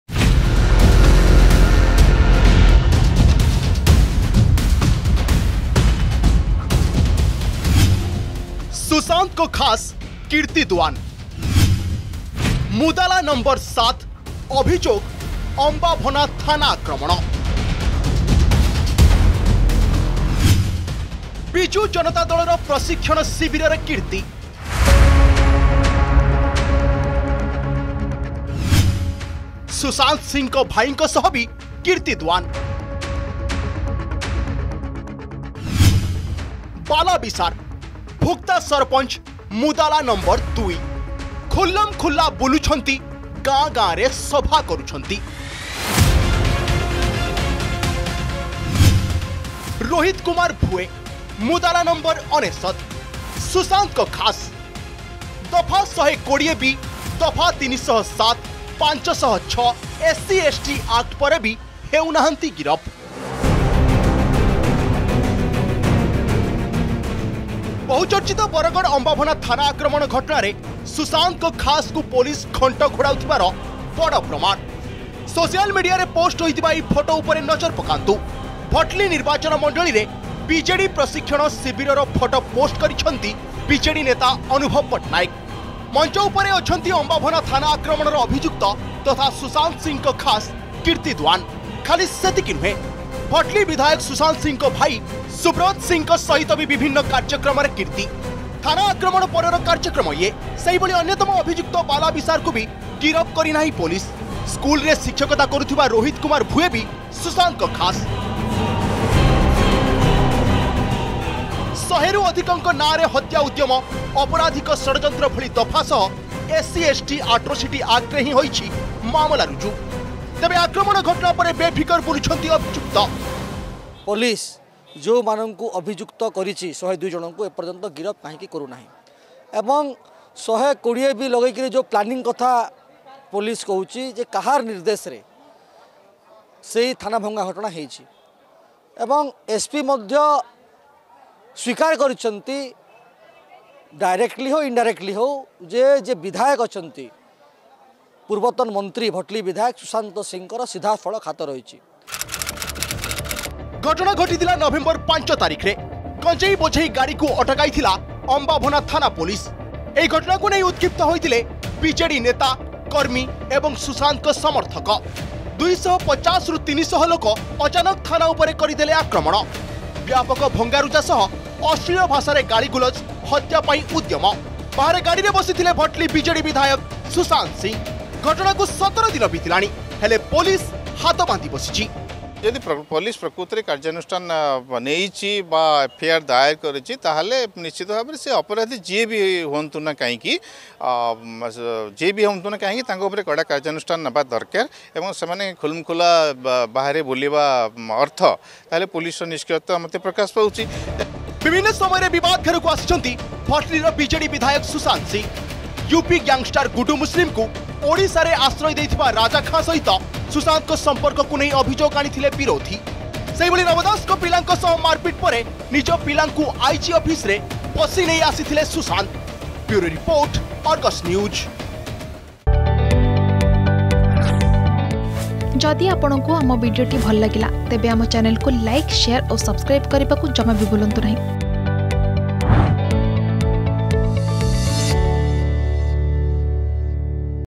ସୁଶାନ୍ତ ସିଂଙ୍କ ଖାସଙ୍କୁ ପୋଲିସ ଘଣ୍ଟ ଘୋଡ଼ାଉଛି ! || BJD || Susanta Singh
ବହୁଚର୍ଚ୍ଚିତ ବରଗଡ଼ ଅମ୍ବାଭୋନା ଥାନା ଆକ୍ରମଣ ଘଟଣା । ବିଜେଡି ବିଧାୟକ ସୁଶାନ୍ତ ସିଂଙ୍କ ଖାସଙ୍କୁ ପୋଲିସ ଘଣ୍ଟ ଘୋଡ଼ାଉଥିବାର ବଡ଼ ପ୍ରମାଣ ଲାଗିଛି ଅର୍ଗସ୍ ନ୍ୟୁଜ୍ ହାତରେ । କିର୍ତ୍ତୀ ଦୁଆନ୍, ବାଲା ବିଶାର, ରୋହିତ ଭୁଏଙ୍କ ବିରୋଧରେ ଲାଗିଛି ସଙ୍ଗୀନ ଦଫା । ଖୁଲମଖୁଲ୍ଲା ବୁଲୁଛନ୍ତି, ଗାଁ ଗାଁ'ରେ କରୁଛନ୍ତି ସଭା । ତଥାପି କାହିଁକି ଗିରଫ କରୁନି ପୋଲିସ ?